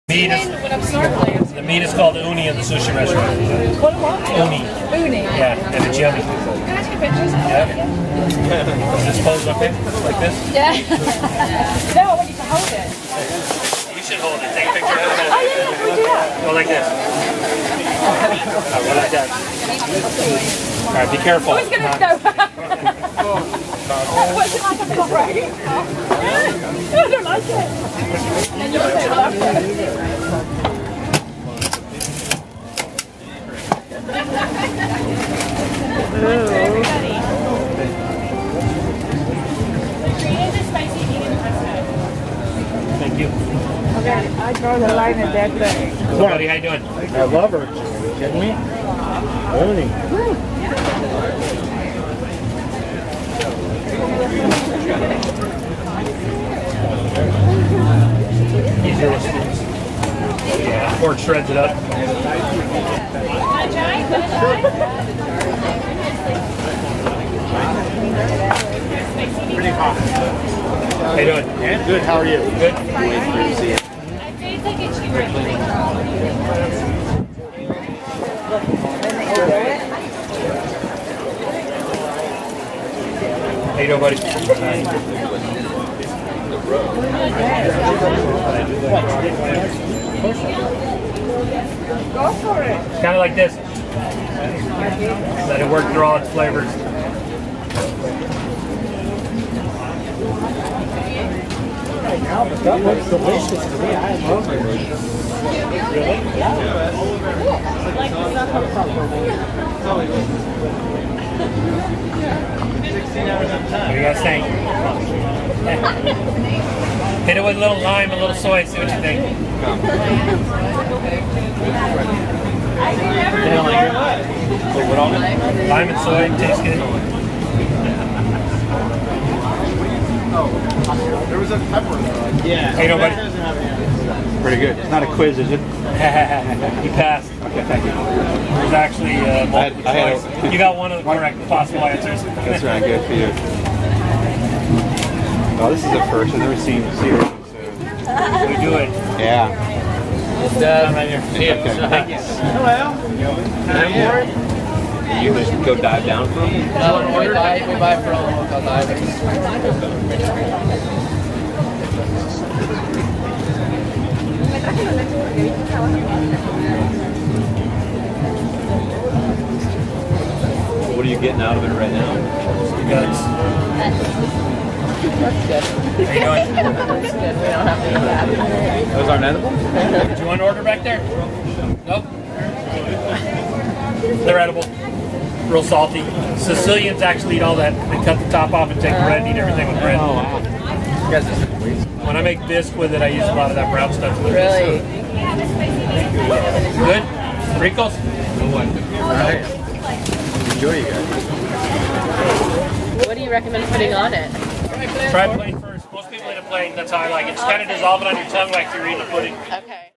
Street Food - Eating LIVE Sea Urchin Uni Sashimi!
Eating live sea urchin sashimi off the street!

At this street stall, sea urchins are still alive when it is cracked open and prepared for the customer.  The uni has a wonderful, creamy texture, and has a fresh sweetness that rival the uni you would find at any sushi restaurant.

Sea urchins feast on kelp, and if you’ve ended up with one who happened to snack on a bitter batch, it could have a slightly bitter, undesirable flavor.  There is no way to physically determine which sea urchins are going to be bitter.  Since the sea urchin is not tasted before given to the customer, there is actually a chance that he or she gets one that will taste bitter!

The sea urchins are served by Poppa's Fresh Fish Company, which is a local San Diego Company dedicated to bringing the freshest seafood to the local farmer's markets.  This video was taken at the Farmer's Market at Little Italy, on a Saturday afternoon.